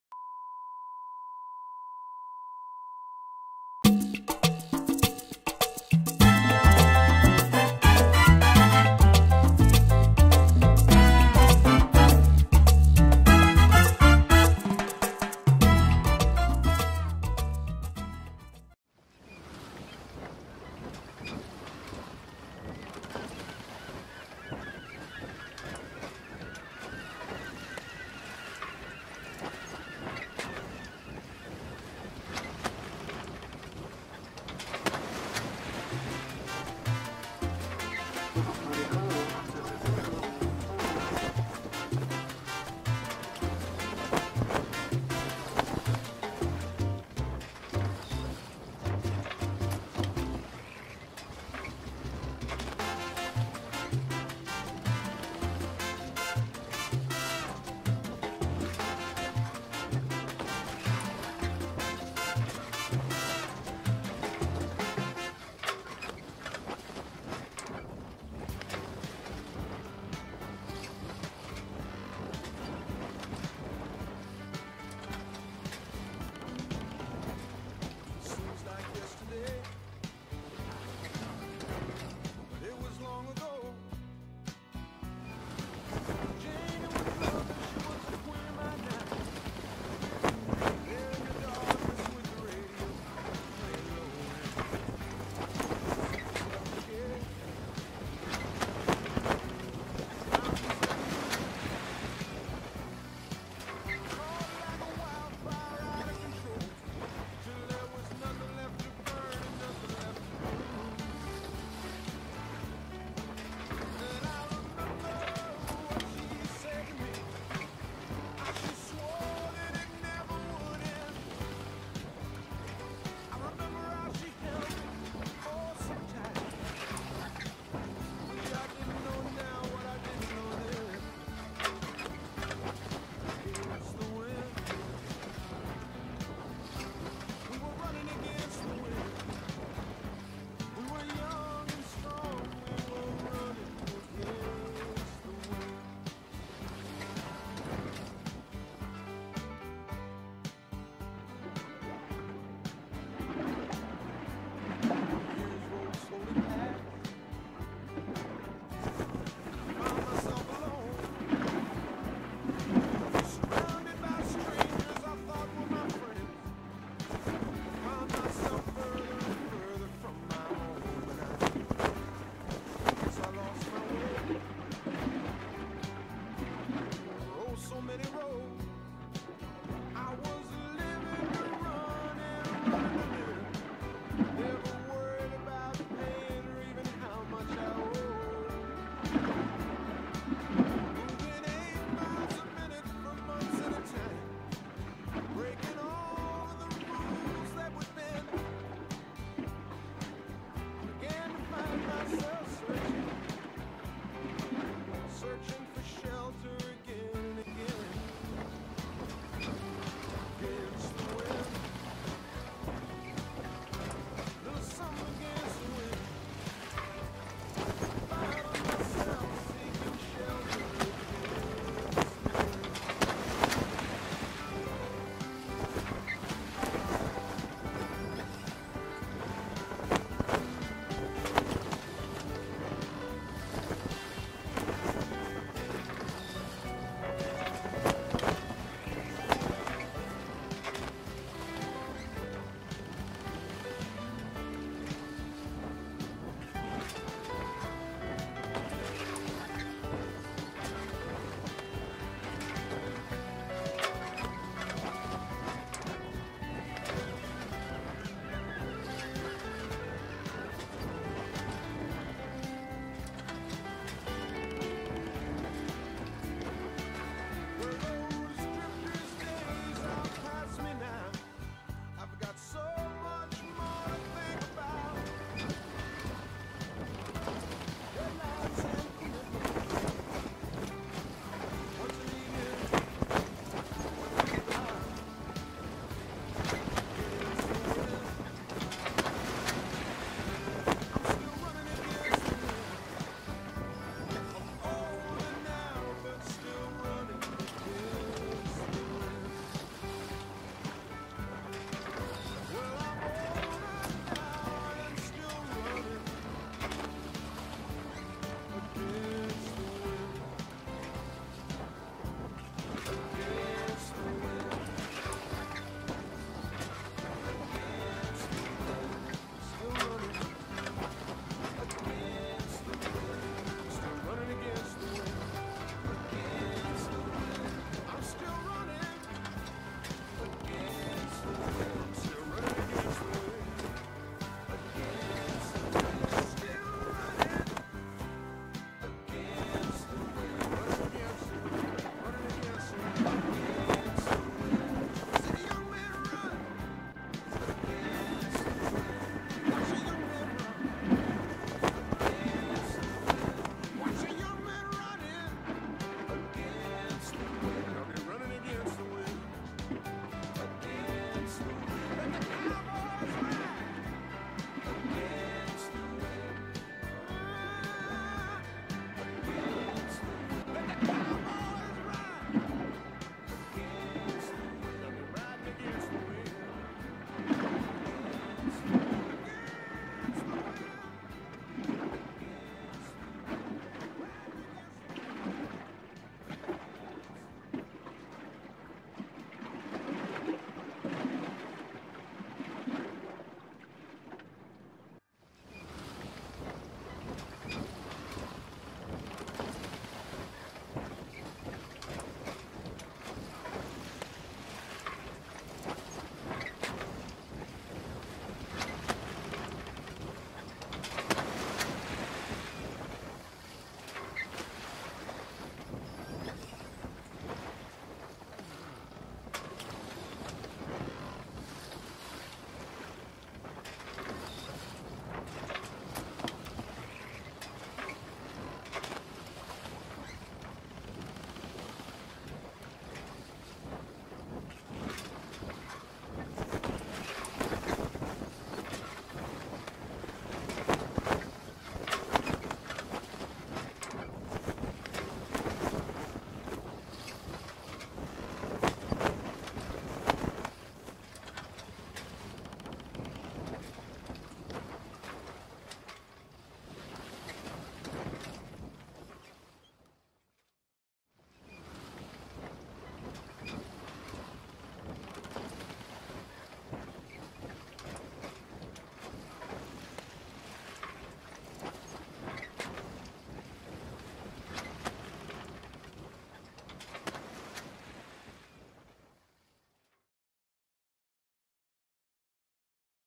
Sailing LunaSea
Sailing my sailboat in Okanagan Lake near Kelowna BC